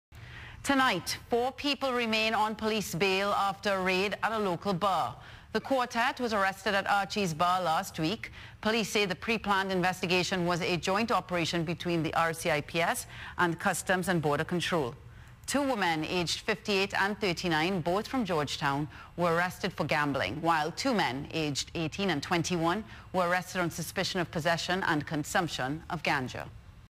UPDATE: Police operation at Archie's
Four people are arrested during a raid at Archie's Bar on Shedden Road. It happened last week.

Police said the pre-planned investigation was a joint operation between the Royal Cayman Islands Police Service and Customs and Border Control.

Two women, ages 58 and 39, both from George Town, were arrested for gambling. While two men, ages 18 and 21, were arrested on suspicion of possession and consumption of ganja.

All four have been released on bail.